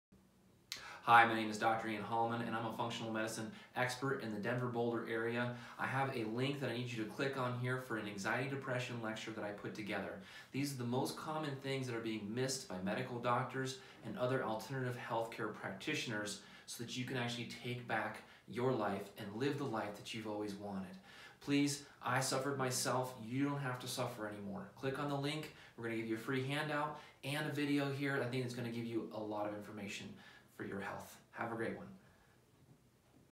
Dr. Ian Introduces his Anxiety/Depression Lecture
Dr. Ian Hollaman, DC, MSc, IFMCP is a nationally recognized speaker on functional medicine and is currently leading a chronic condition practice in Boulder, CO.